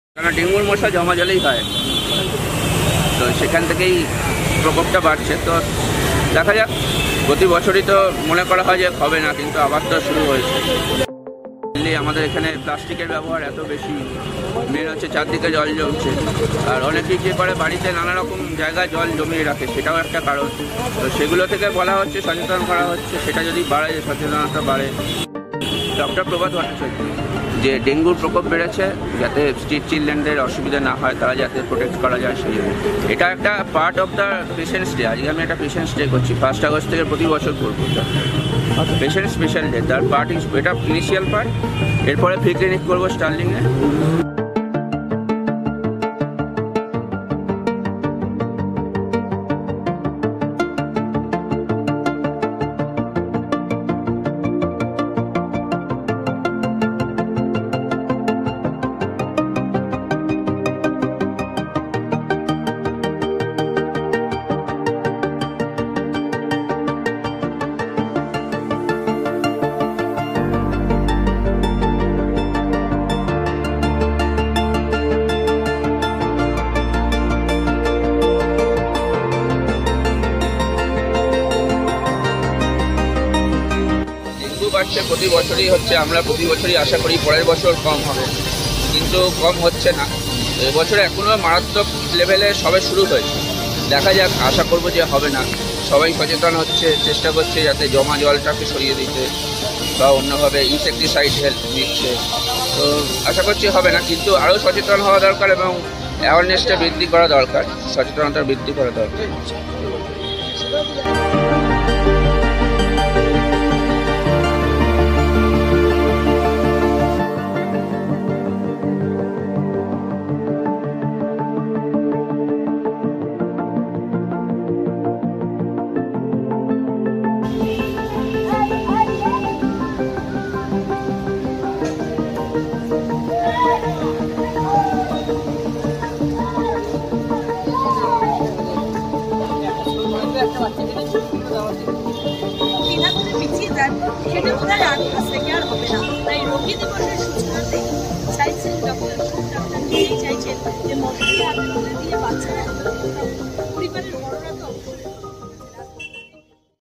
West Bengal Dengue cases || ডেঙ্গু থেকে বাঁচাতে মশারি হাতে ঘুরলেন ডাক্তার || ieBangla
রাজ্যজুড়ে ডেঙ্গুর বাড়বাড়ন্তে পথশিশুদের সুরক্ষিত রাখতে মশারি হাতে পথে নামলেন ডাক্তারবাবু। চিকিৎসক প্রভাত ভট্টাচার্য। ডাক্তারবাবু নিজেই রাস্তায় শুয়ে থাকা পথ শিশুদের ওপর মশারি টাঙিয়ে দিচ্ছেন। হাতে স্টেথো। সঙ্গে বার্তা, 'কম্বল তো আকছার দেওয়া হয়। মশারি দিন। বাঁচবে প্রাণ।' এভাবেই এই ডাক্তারবাবু পালন করছেন রোগী দিবস।